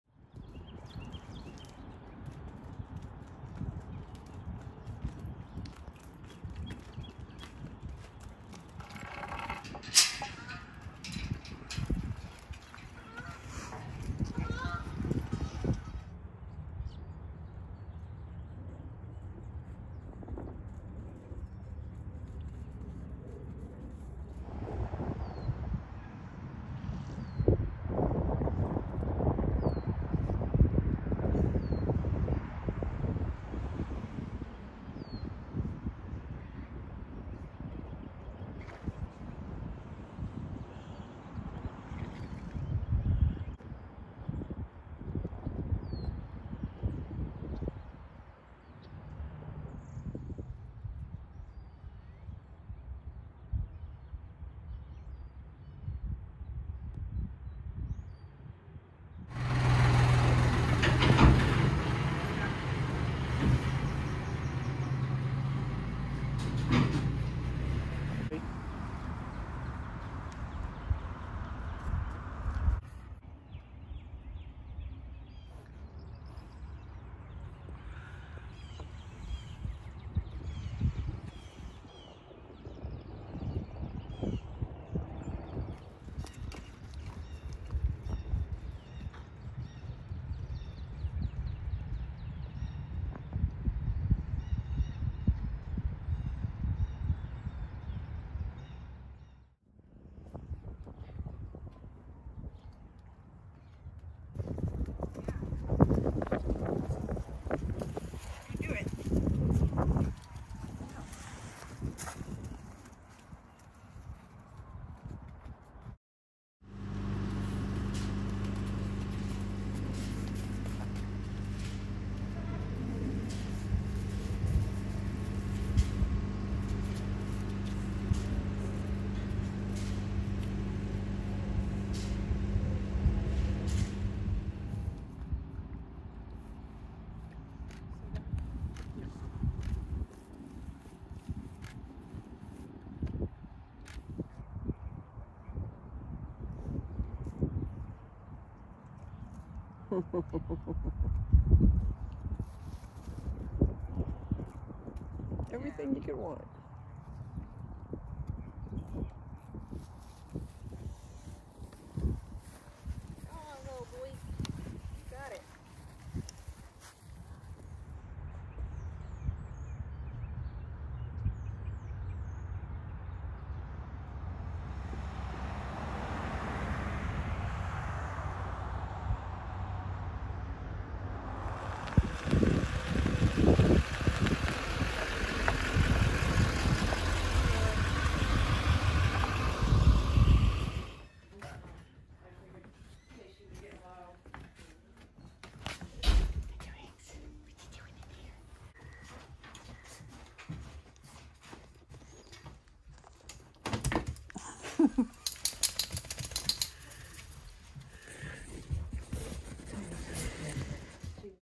Country Life Farm week 1, thru 3-16-20
A series of clips from life at Country Life ( first week of Covid 19 closures ) captured by Ellen Pons